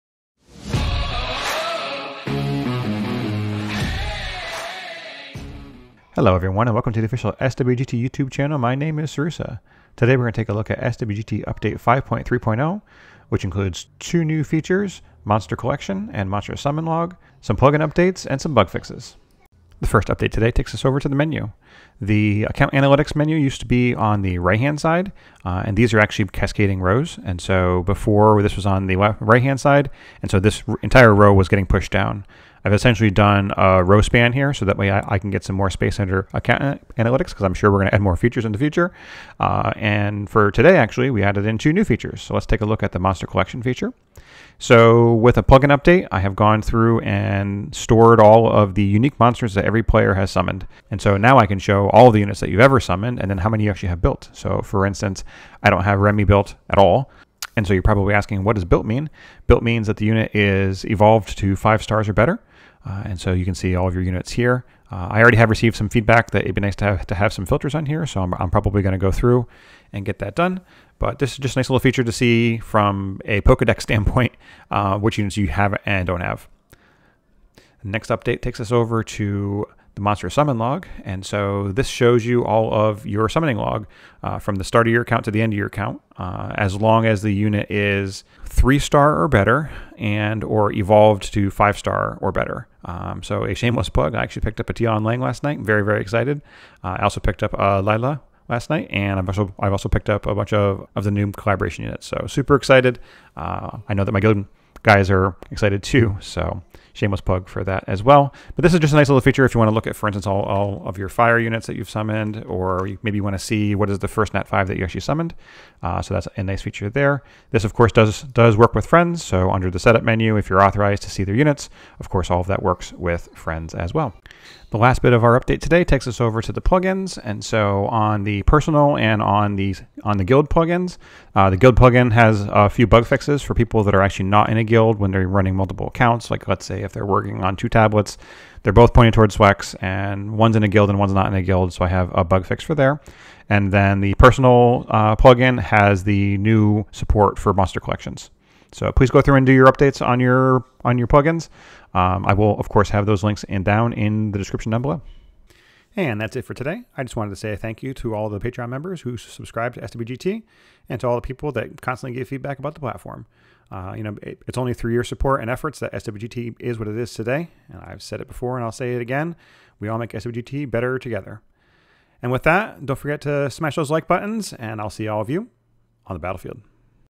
SWGT - Episode 20 - Update 5.3.0 - Monster Collection & Summon Log, Plugin updates and bug fixes
In this episode, we'll cover the SWGT Updates 5.3.0 which rearranges the SWGT Menu, adds a two new features (Monster Collection and Monster Summon Log), some plugin updates and bug fixes.

Chapters:
0:00 Intro
0:06 What is this video about?
0:19 Updated SWGT Menu
0:43 New Monster Collection feature
1:16 New Monster Summon Log feature
2:05 Plugin updates and bug fixes
2:35 Outro